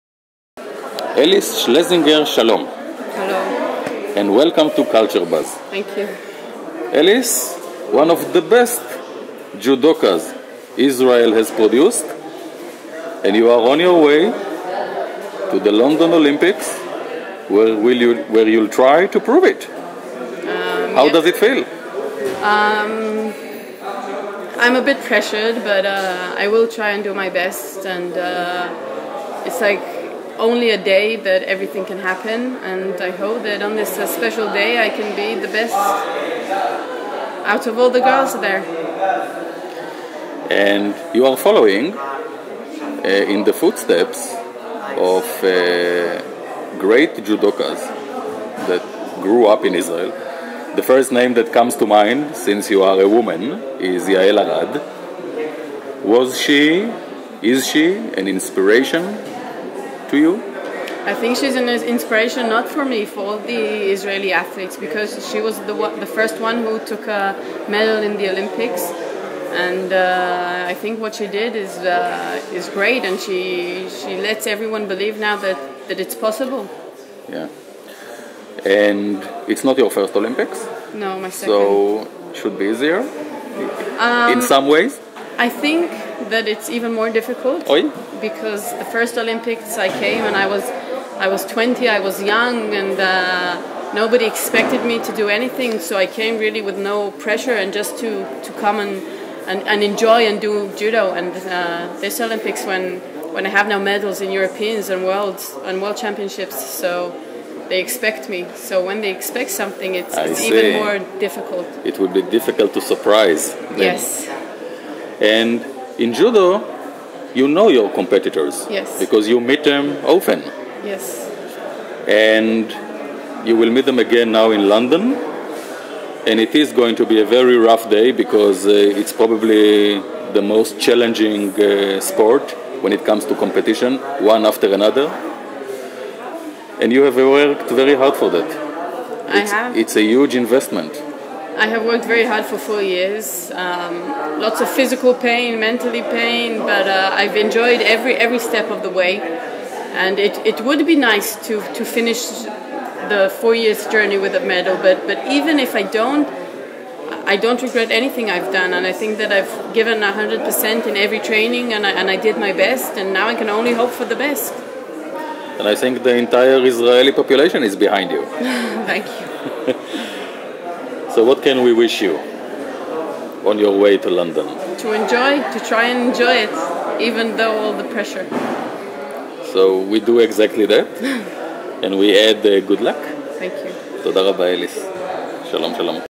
CultureBuzz Converses with Alice Schlesinger - Just before leaving for her second Olympics!
Young but already an accomplished Judoka on the highest International level, Alice demonstrates here how she became so determined, focused & successful.

How tough it's gonna be? Why's the 2nd Olympics more difficult than the 1st one? Find out here!

Inspiring indeed Alice and all the best at your second Olympics!

Special thanks to Bruria Bigman & for arranging this.